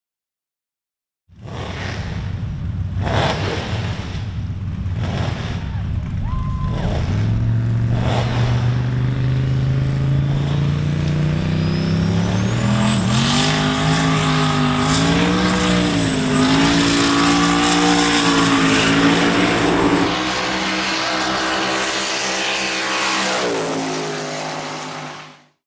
2SHOWOFF - POWERSKID WINNER AT WEST COAST NATS
Jamie takes the Powerskid win at West Coast Nats in his VE GTS "2SHOWOFF" and goes deep into the sand trap with the chute pulled.  Running a 403 Cube LS with a 94mm Turbo this weapon recently pushed out 1,405rwhp.